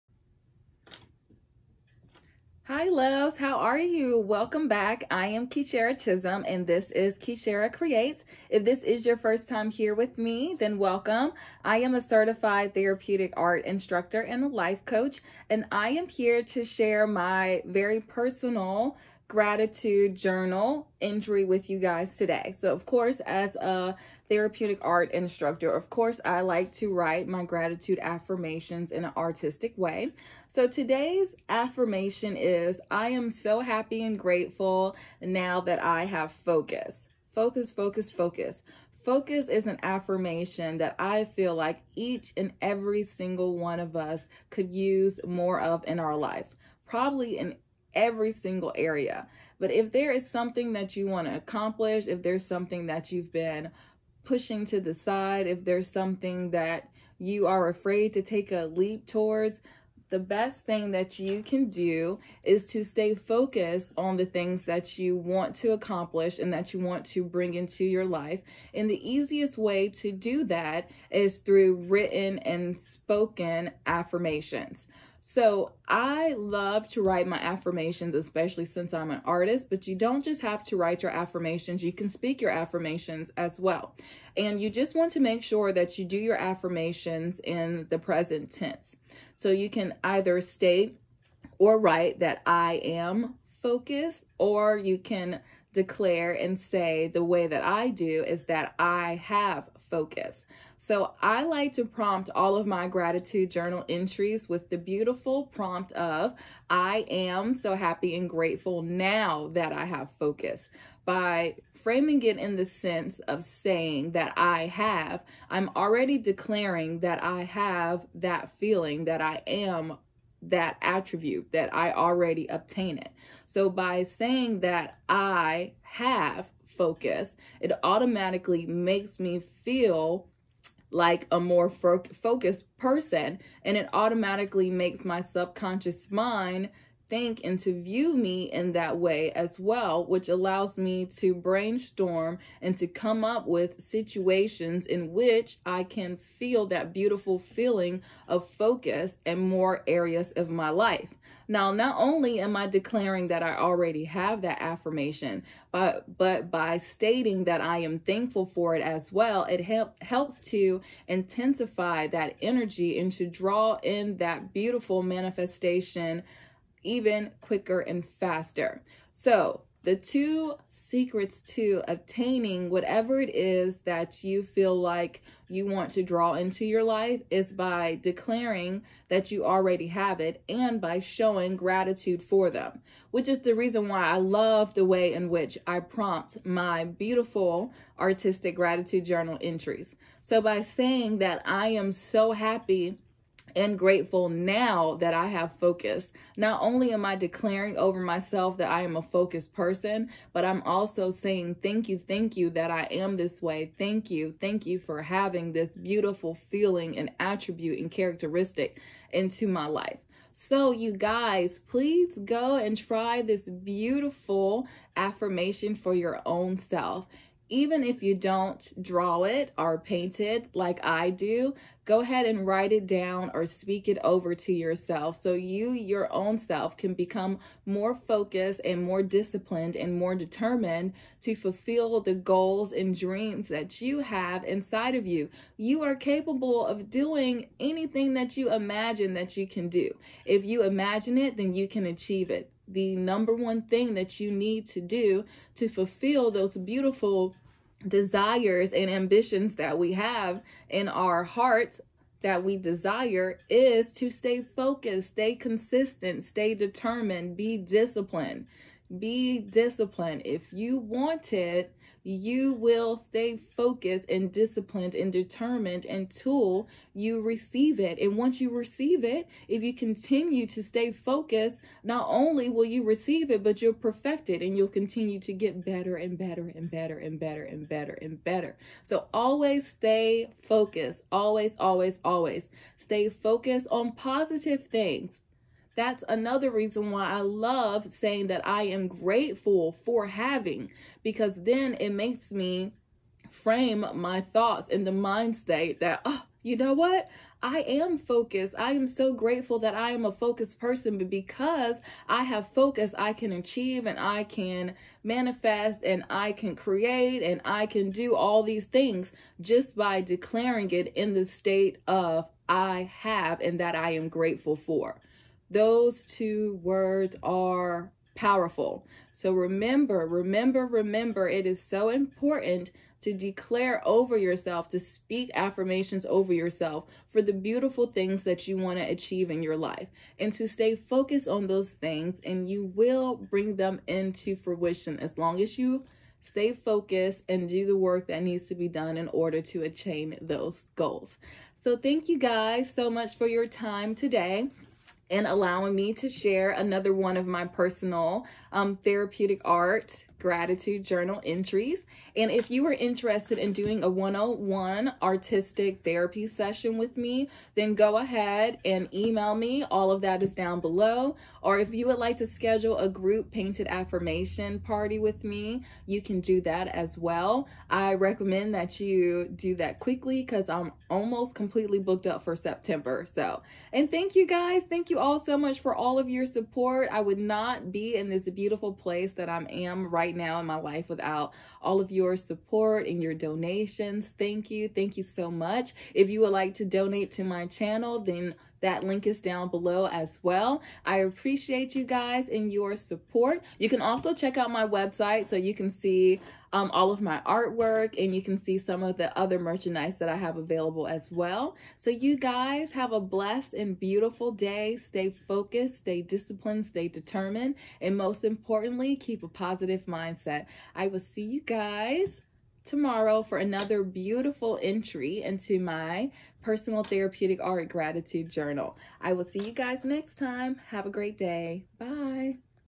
Art Journaling For Gratitude Therapeutic Art Activity
Hello, beautiful creators. Welcome back for another therapeutic art activity. I wanted to share with you all a personal activity that I use for manifestation with art journaling. Art journaling for gratitude is a beautiful way to draw in positive energy and helps you to keep your focus on all the beautiful things to show gratitude for. Today’s art journaling for gratitude entry is life. We can all show gratitude for life. Art journaling is a fun and beautiful way that you can incorporate therapeutic art and creative healing in your daily life. When I begin my day with gratitude journaling it helps me to keep my mind focused on positivity and all the wonderful things that I have to be grateful for. This therapeutic art journaling activity is perfect for beginners and artists of all experience levels. Art journaling is a simple and therapeutic way to show and share gratitude in a personal and creative way. I hope that this art journaling for gratitude therapeutic art activity brings you just as much joy as it did for me.

Thank you for joining me today as I shared my first art journaling for gratitude therapeutic art entry with you. I appreciate each of you. Thank you in advance. It truly helps my channel to grow. Be blessed!!!